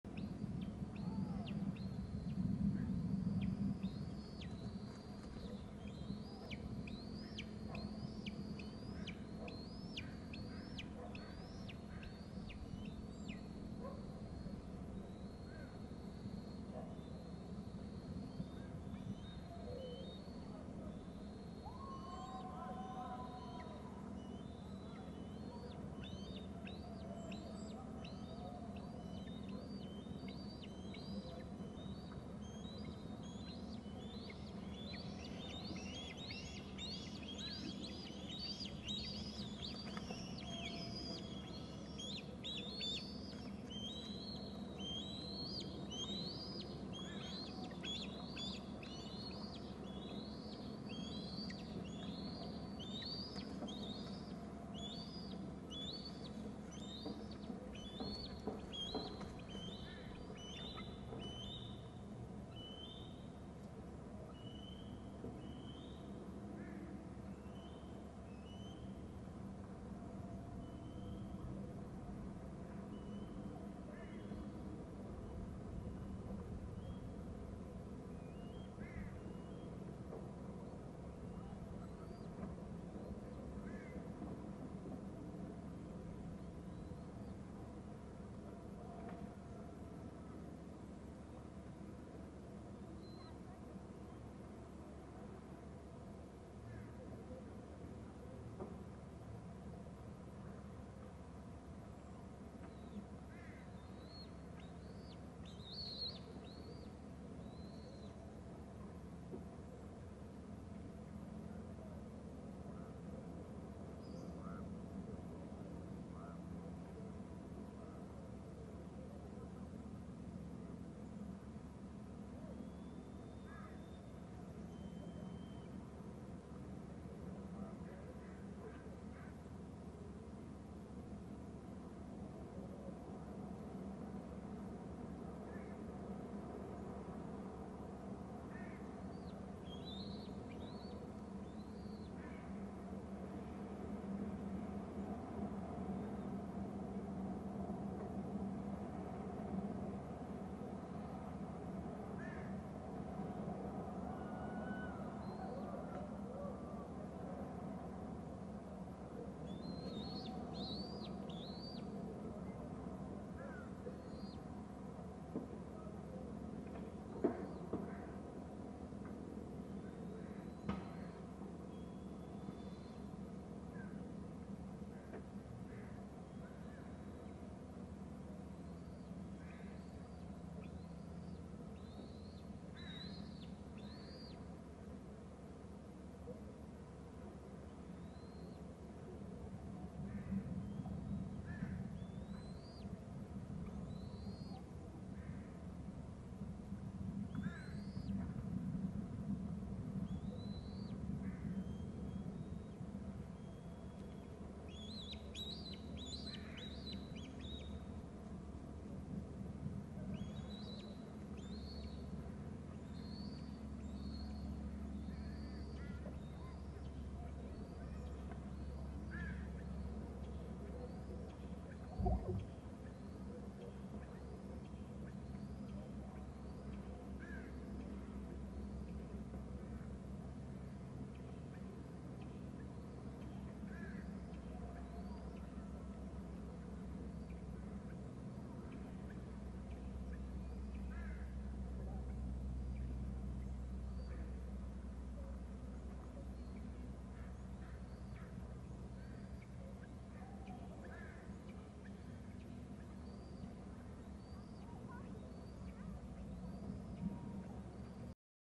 13.44 eaglet off bst to the left lands on nest tree
13.44 eaglet off bst to the left lands on nest tree